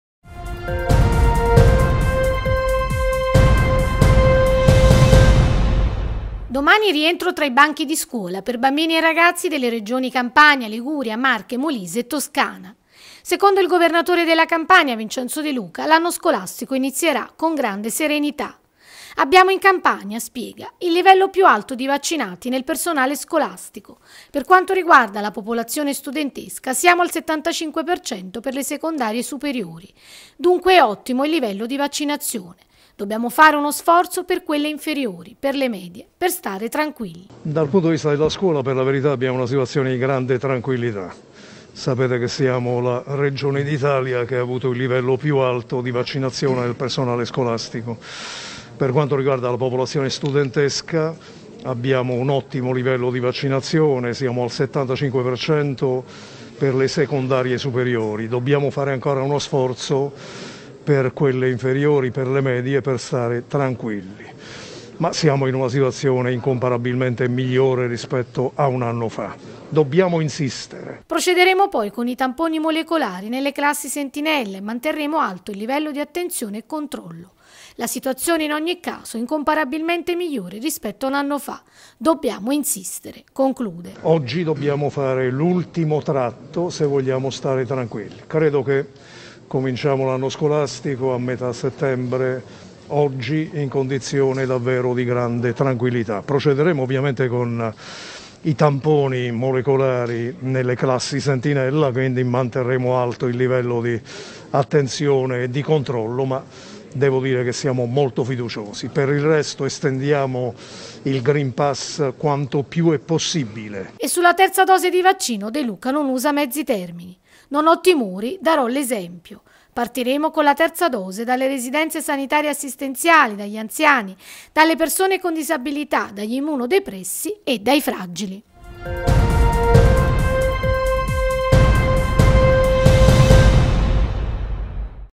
De Luca sul rientro a scuola: "Situazione migliore rispetto a un anno fa"
Domani rientro tra i banchi di scuola per bambini e ragazzi delle Regioni Campania, Liguria, Marche, Molise e Toscana.
Secondo il governatore della Campania Vincenzo De Luca, l’anno scolastico inizierà con grande serenità.
"Abbiamo in Campania - spiega - il livello più alto di vaccinati nel personale scolastico. Per quanto riguarda la popolazione studentesca, siamo al 75% per le secondarie superiori, dunque è ottimo il livello di vaccinazione. Dobbiamo fare uno sforzo per quelle inferiori, per le medie, per stare tranquilli.
Procederemo con i tamponi molecolari nelle classi sentinella e manterremo alto il livello di attenzione e controllo. La situazione, in ogni caso, è incomparabilmente migliore rispetto a un anno fa. Dobbiamo insistere”, conclude.
E sulla terza dose di vaccino, De Luca non usa mezzi termini.
“No ho timori, darò l’esempio. Partiremo, con la terza dose, dalle residenze sanitarie assistenziali, dagli anziani, dalle persone con disabilità, dagli immunodepressi e dai fragili”.